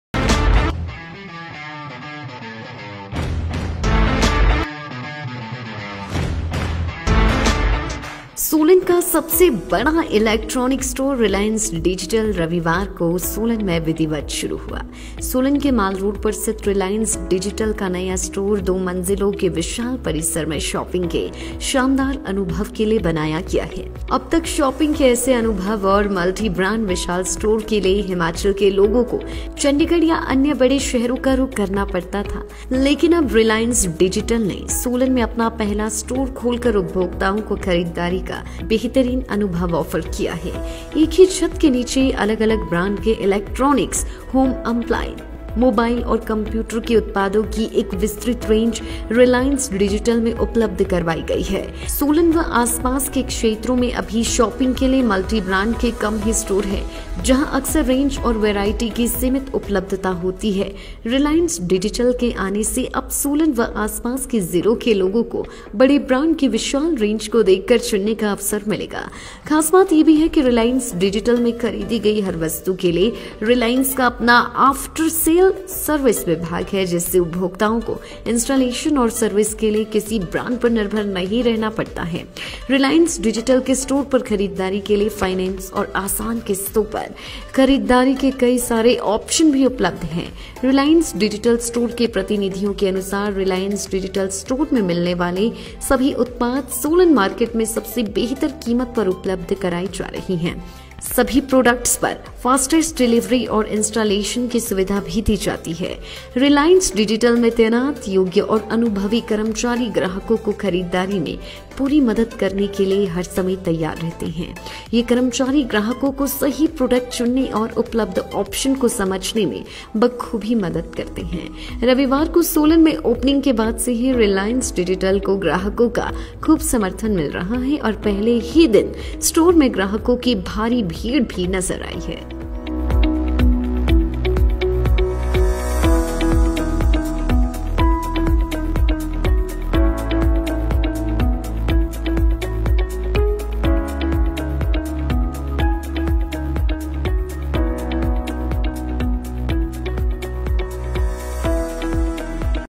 हिमाचल प्रदेश के सोलन शहर में खुला है सबसे पहला-रिलायंस डिजिटल स्टोर..- शानदार ऑफर्स और...
हिमाचल प्रदेश के सोलन शहर में खुला है सबसे पहला-रिलायंस डिजिटल स्टोर..
- शानदार ऑफर्स और फाइनेस की भी सुविधा...